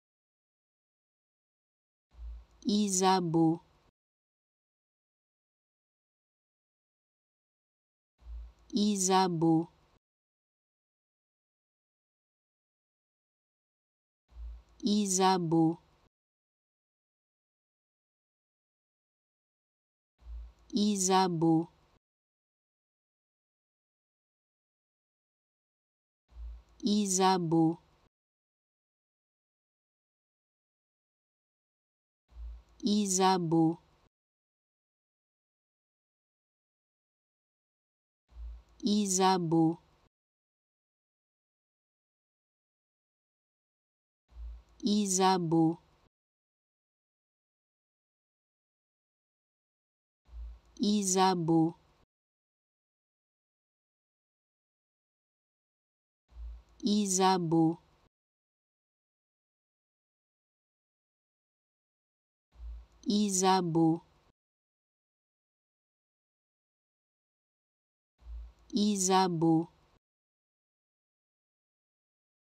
How to Pronounce ''Ysabeau'' Correctly in French
Learn how to say and properly pronounce ''Ysabeau'' in French with this free pronunciation tutorial. Apprenez à prononcer des mots en français grâce à nos tutoriels.